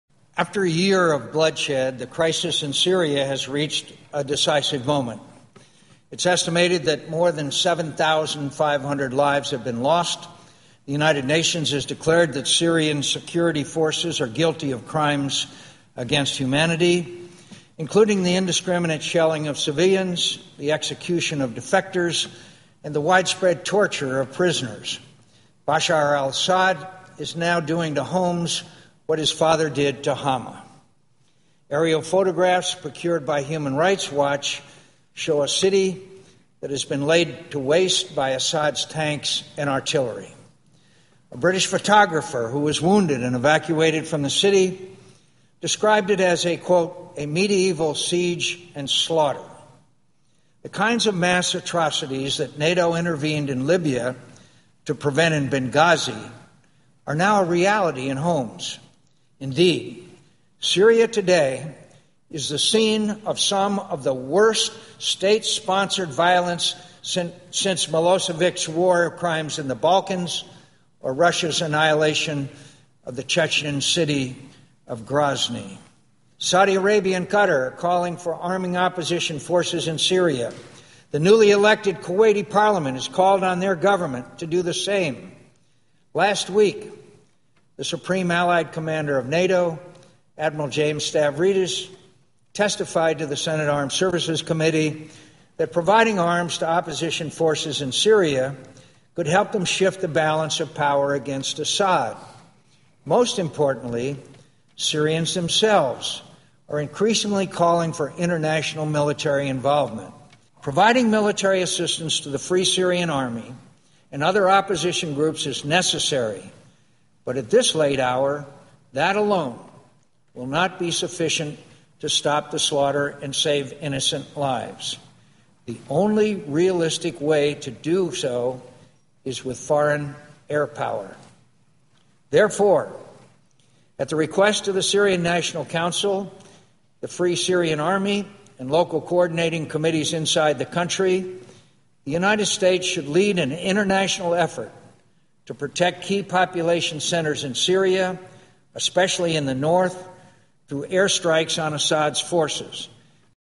McCain calls for US Airpower in Syria
Senator John McCain calls for an end to "the slaughter of innocent lives" in Syria through a US-led international effort to protect the population while launching airstrikes on Assad forces.